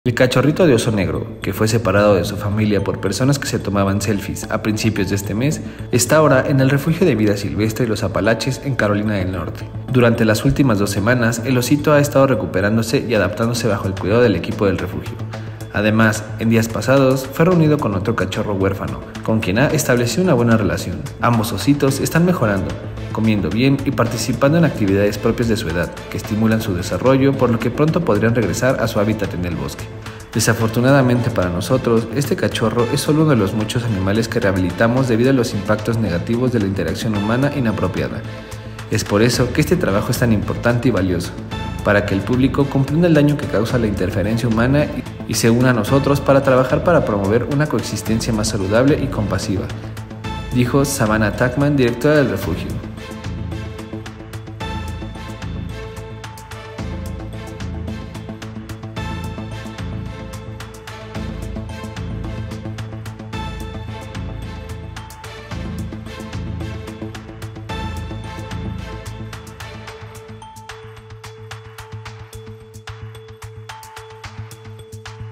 Osito arrebatado de su familia por personas que buscaban una selfie
El cachorrito de oso negro, que fue separado de su familia por personas que se tomaban selfies a principios de este mes, está ahora en el Refugio de Vida Silvestre de los Apalaches en Carolina del Norte.